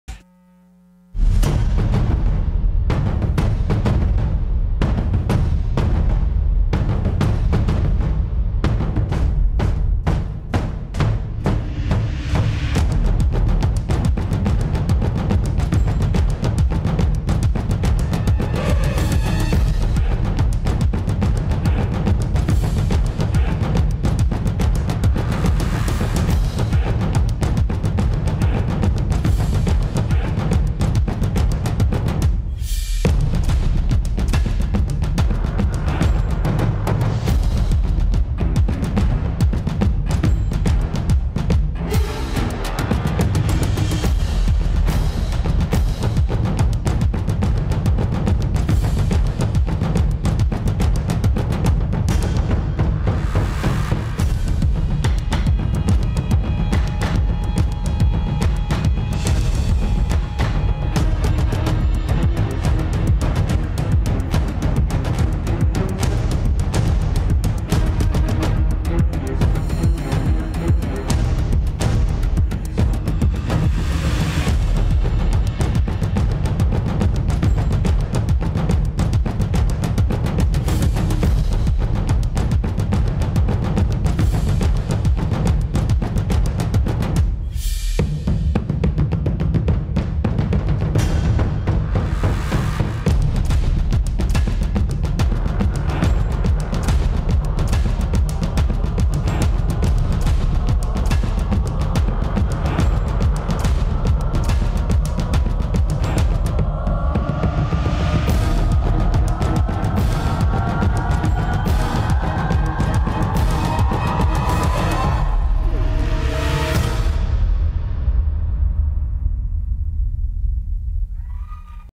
Ruby Castro - Drums
Age 8
Contemporary Solo
Choreography by Manny and Lory Castro
DanceTown
2012